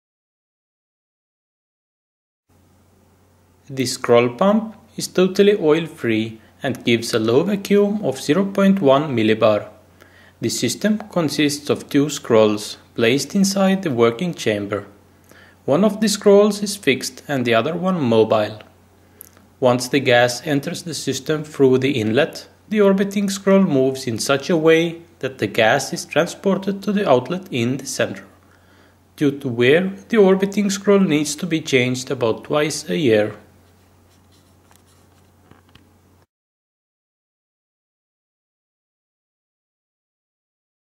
Scroll Pump | Vacuum Pumps
Scroll pump (scroll compressor) is an oil-free vacuum pump used for obtaining pre vacuum with a level of 0.1 mbar. The scrollpump is based on two scrolls, that are placed insede the working chamber. One of these scrolls is fixed and the other one mobile. Once the gas enters the system through the inlet, it gets carried to the outlet in the middle by the mobile orbiting scroll. This is possible due to the fact that during the mobile scrolls movement a wide gap is locally kept between the two scrolls and the gap moves to the middle, carrying the gas in it. As the volume of the gap decreases at the outlet, the gas is pushed out of the system. Due to wear the orbiting scroll needs to be replaced about twice a year.